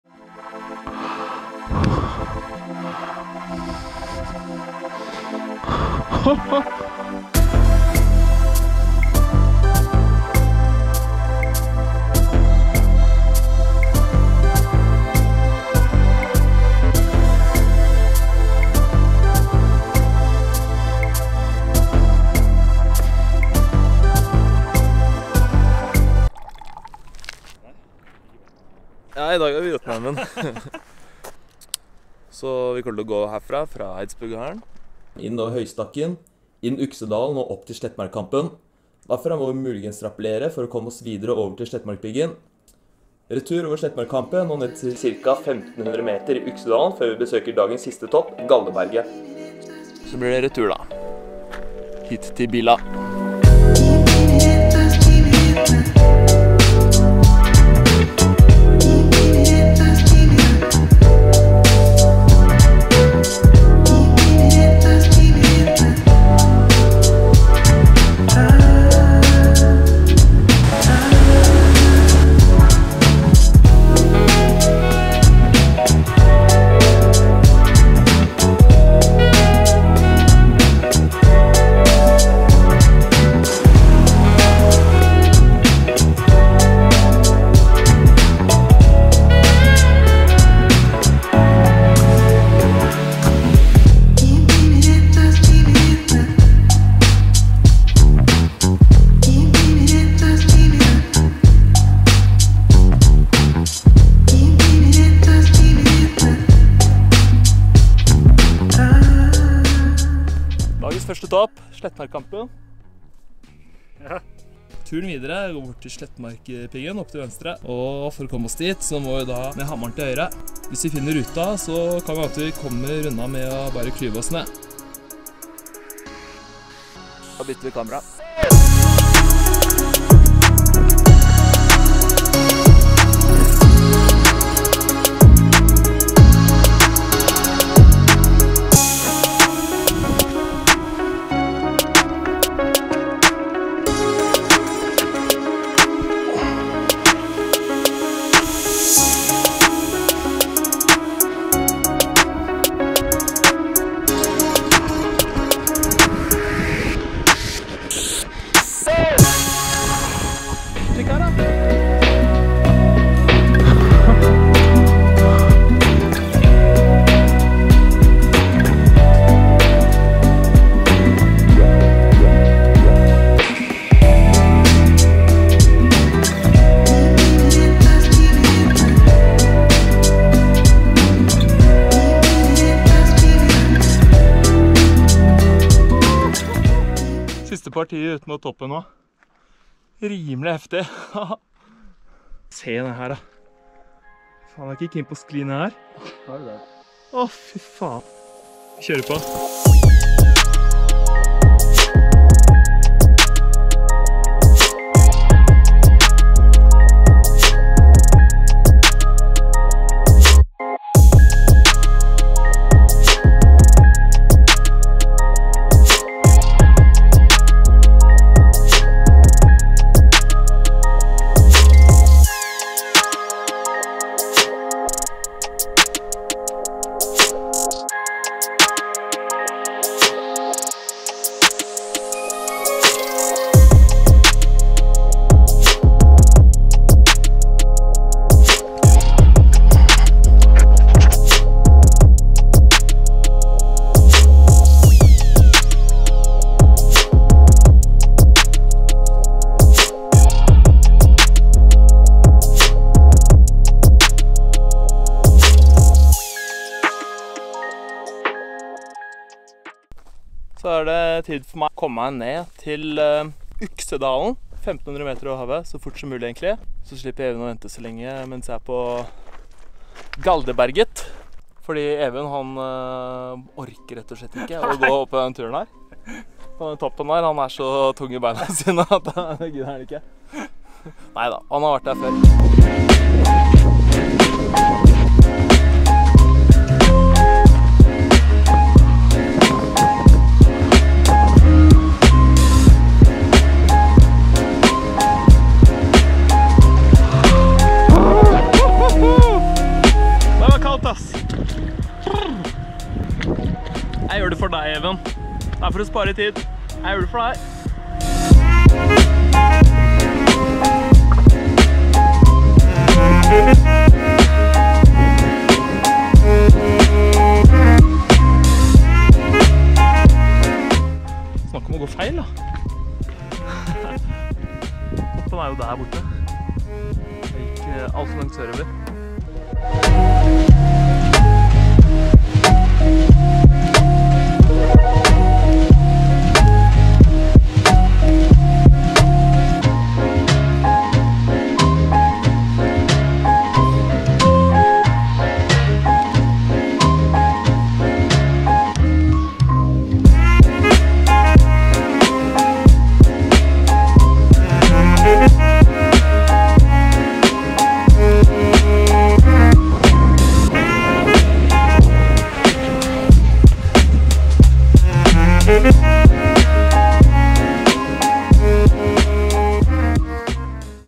Spenstig toppunkt på SLETTMARKPIGGEN -//Jotunheimen
Knallfin tur fra Fondsbu/Eidsbugarden til Slettmarkpiggen og Galdeberget med Even. Artig nedklyving fra Slettmarkhø før man entrer den eksponerte hylla mot toppartiet, etterfulgt av klying opp til toppunktet.

Gikk deretter opp til Galdeberget før retur til Fondsbu/Eidsbugarden. Total distanse 29.4km og 1840 høydemeter. Var ute i 9 timer. Anbefales!

Filmet med Sony a6600, GoPro Hero 8. Redigert i FCPX.